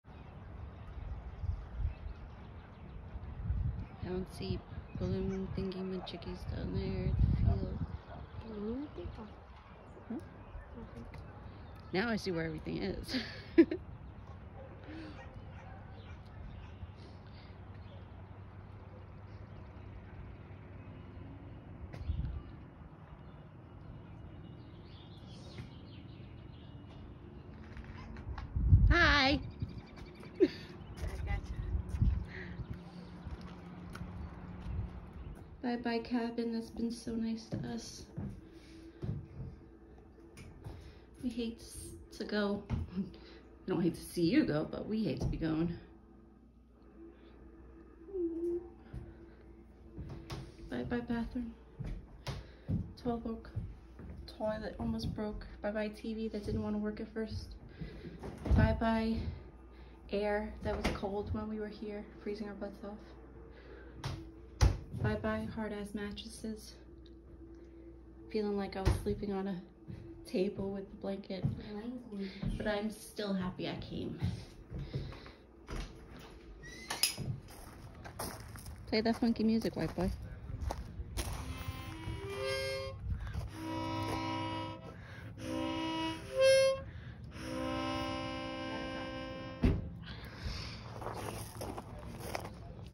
Yogi Bear's JellyStone Park, Gardener NY, April 2021
Last Day camping. So sad we have to go! Until next time Yogi!